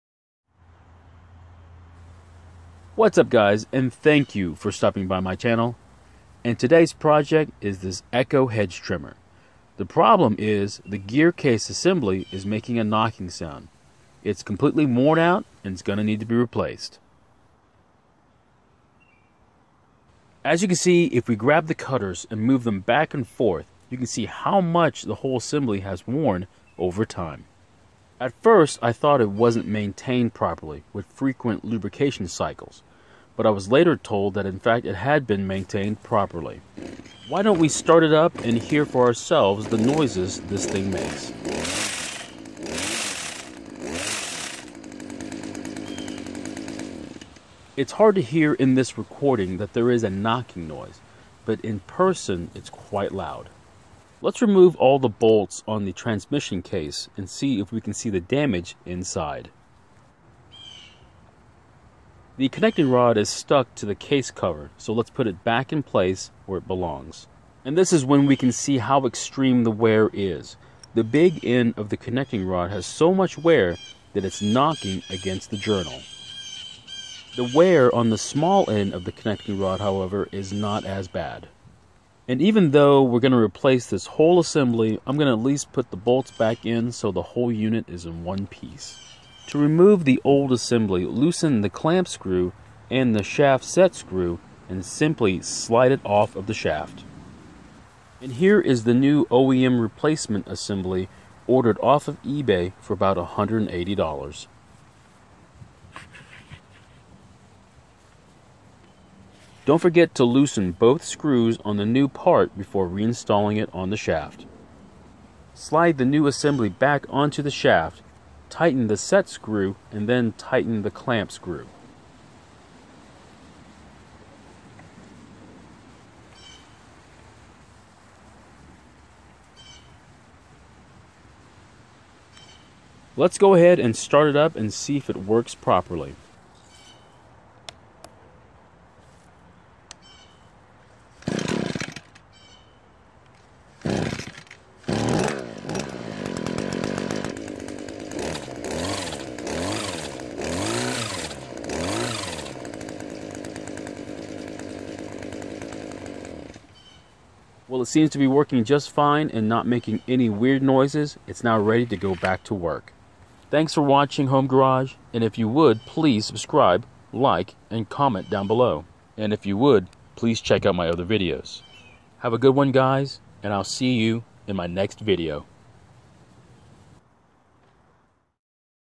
Echo hedge trimmer problem
Spark Checker.
Tygon Fuel Line.
Carburetor Adjustment tools.
Primer Bulb.
Fuel Filter.

Extreme wear is the cause for this problem.Time to replace the hedge trimmer section.  The part was on Ebay for a reasonable $179.